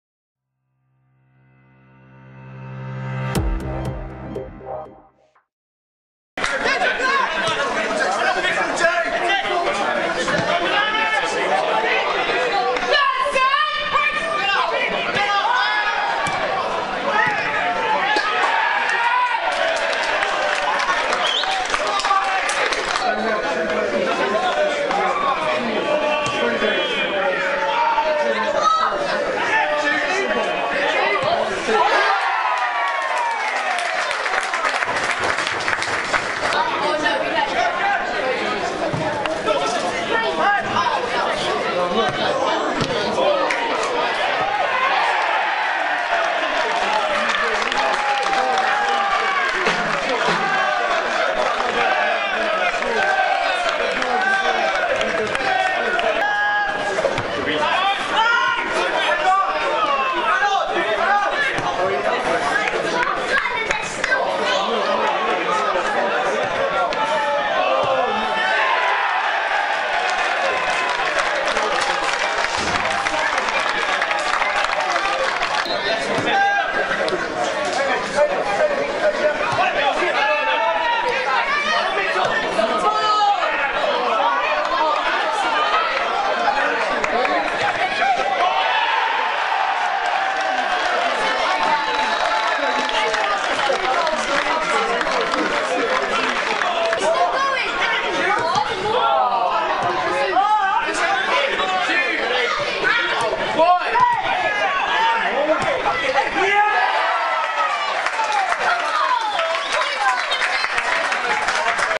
Last Time Out: Boreham Wood 2-4 Macclesfield
See the goals from our 4-2 win over Boreham Wood from earlier in 2017 as Mitch Hancox stars for John Askey's side